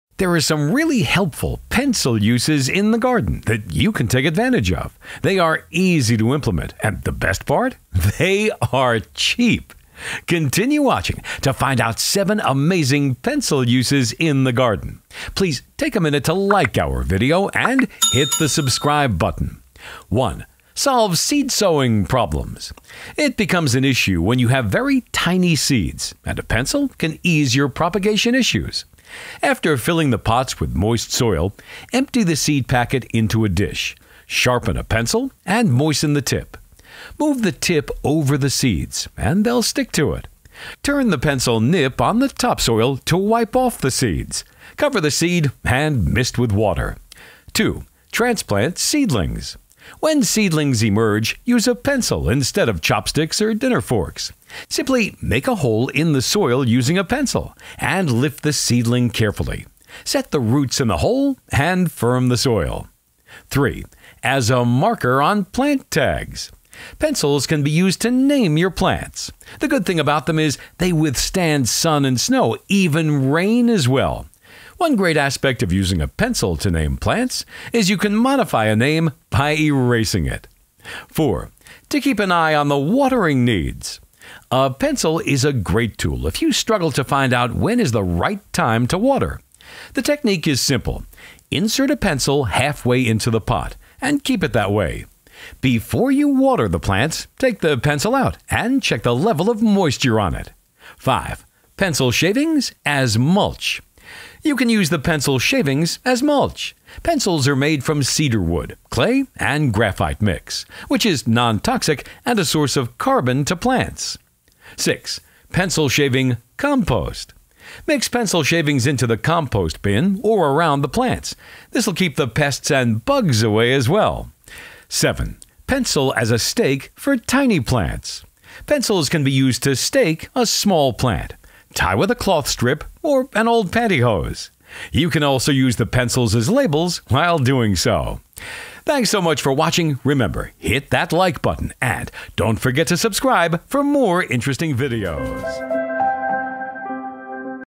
7 Amazing Pencil Uses in the Garden
There are some really helpful Pencil Uses in the Garden that you can take advantage of! They are easy to implement and the best part—they are cheap!

1. Solve Seed-Sowing Problems
Pencil Uses in the Garden
It becomes an issue when you have very tiny seeds and a pencil can ease your propagation issues. After filling the pots with moist soil, empty the seed packet into a dish, sharpen a pencil and moisten the tip.
Move the tip on seeds, and they will stick to it. Turn the pencil nip on the topsoil to wipe off the seeds. Cover the seed and mist with water.

2. Transplant Seedlings
When seedlings formed, use a pencil instead of chopsticks or dinner forks. Simply make a hole in the soil using a pencil and lift the seedling carefully. Set the roots in the hole and firm the soil.

3. As a Marker on Plant Tags
Pencils can be used to name your plants—the good thing about them is they withstand sun and snow, even rain as well! One great aspect of using a pencil to name plants is you can modify a name by erasing it.

4. To Keep an Eye on the Watering Needs
There are a number of ways that will help you while keeping an eye on the watering needs of plants. A pencil is also a great tool that can make the task much easier for knowing when to water your plant.
The technique is simple—insert a pencil halfway into the pot and keep it that way. Before you water the plants, take out the pencil and check the level of moisture on it.
We have a great article on how to use a pencil for watering plants here
5. Pencil Shavings as Mulch
You can use the pencil shavings as mulch. Pencils are made from cedar wood, clay, and graphite mix which is non-toxic and a source of carbon to plants.

Note: Using pencil shaving as a mulch can block the airflow and catches too much moisture that can lead to root rot and fungus. To neutralize this issue, fluff and rake the pencil shaving mulch once in a while.

6. Pencil Shaving Compost
Mix pencil shavings into the compost bin or around the plants—this will keep the pests and bugs away as well.

7. Pencil as a Stake for Tiny Plants
Pencils can be used to stake a small plant, tie with a cloth strip, or an old pantyhose! You can also use the pencils as labels while doing so.